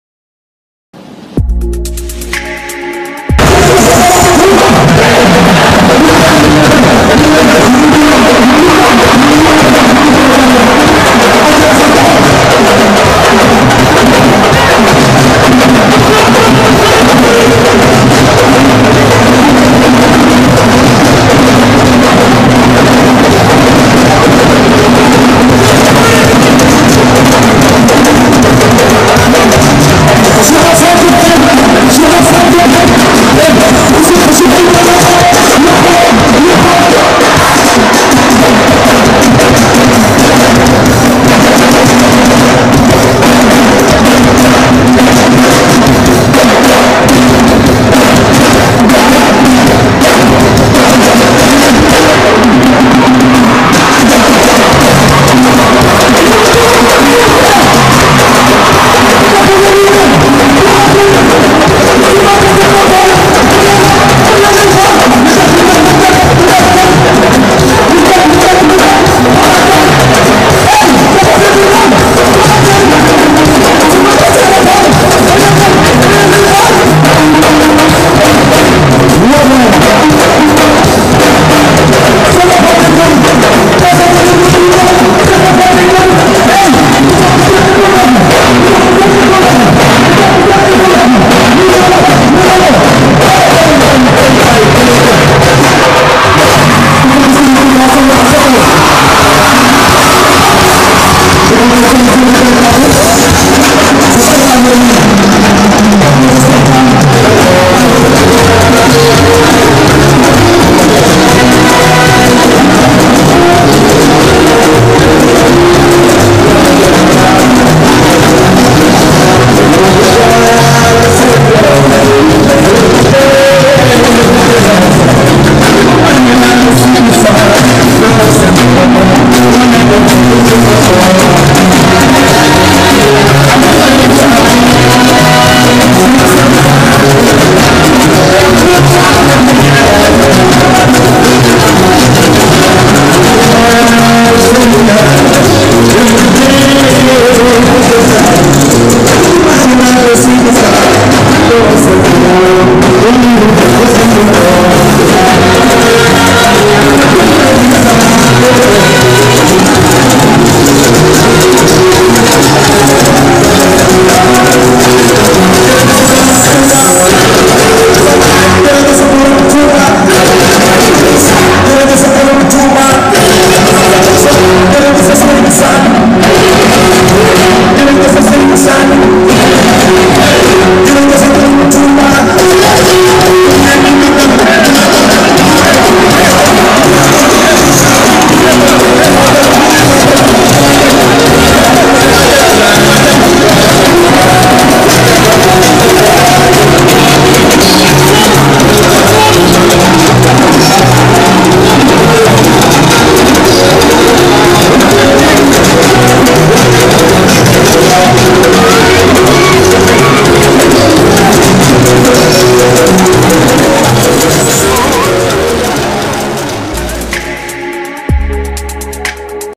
Grand Bégué: Pape Diouf enflamme le Grand Théâtre
Pape Diouf enflamme le Grand Théâtre lors de son Grand bégué